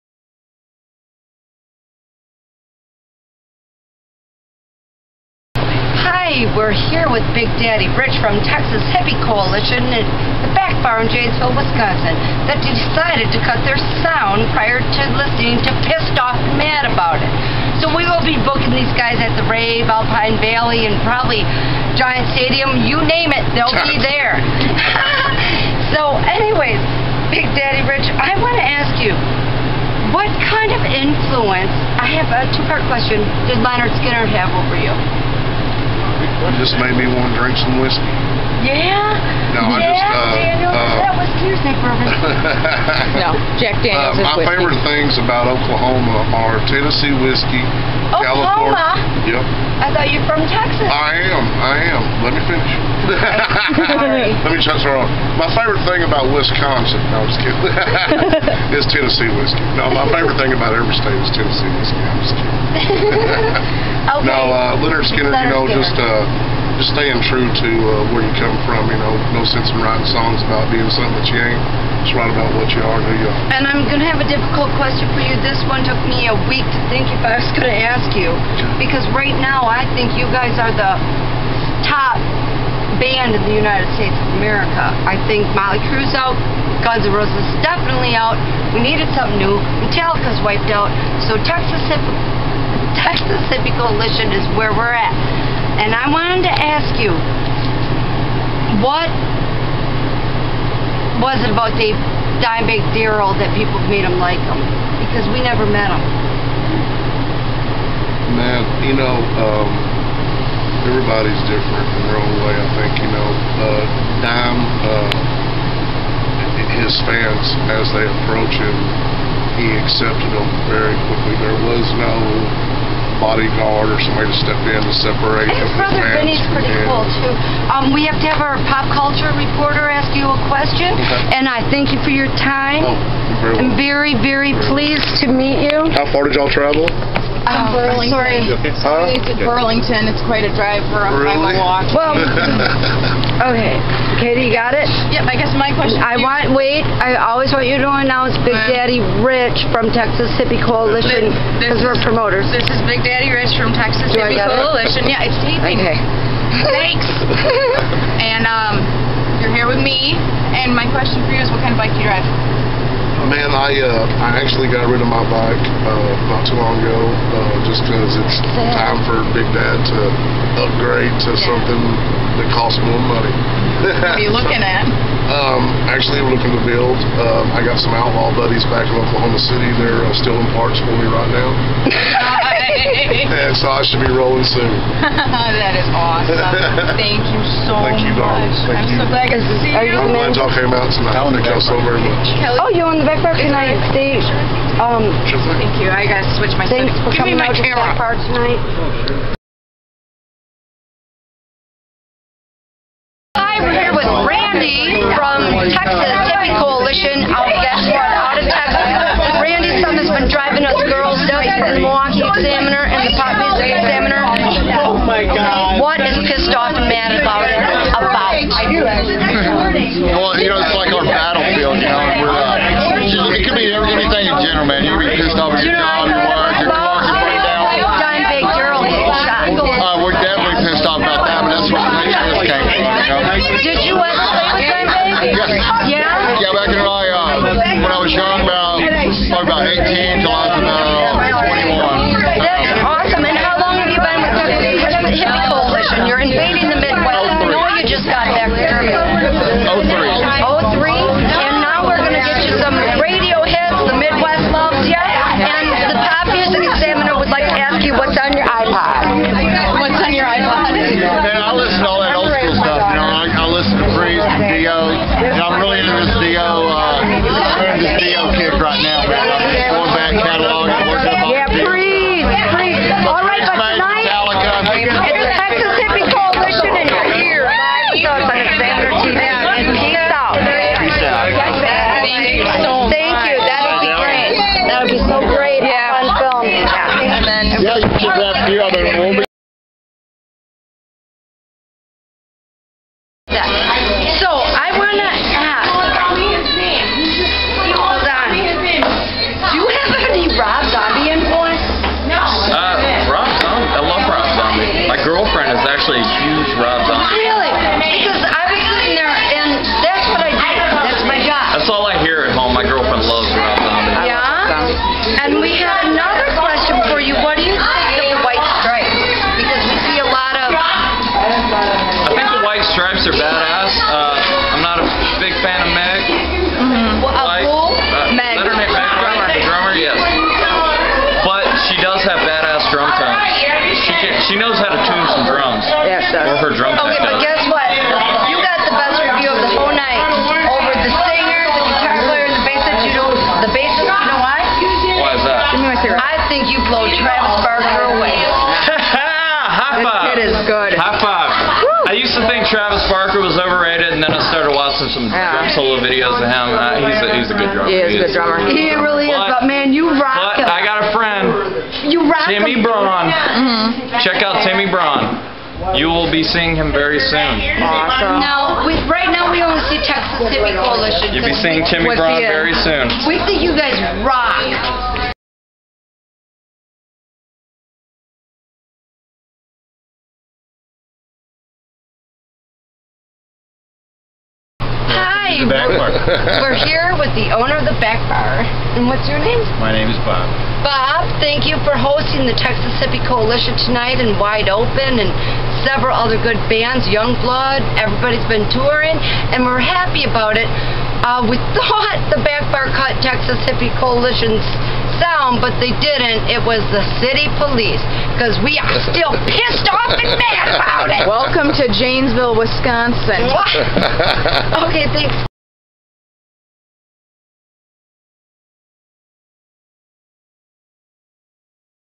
Kidkel69 Interviews 3 Members of Texas Hippie Coalition
Texas Hippie Coalition at the Back Bar in Janesville, WI
Kellie Levans, Milwaukee Rock Examiner
Rocking Fun Music/ All Most Famous Productions